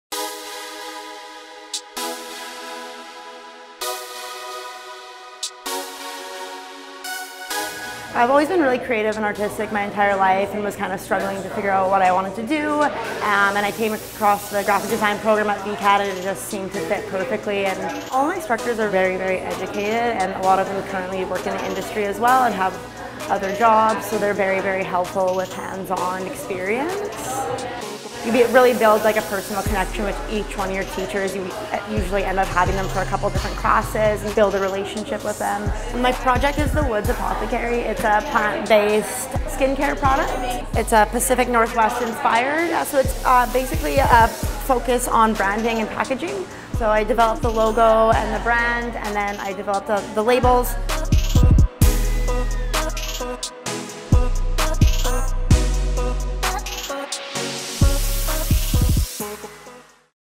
Hailey - Graphic Design Grad
Hailey discusses the training she received from industry-experienced instructors.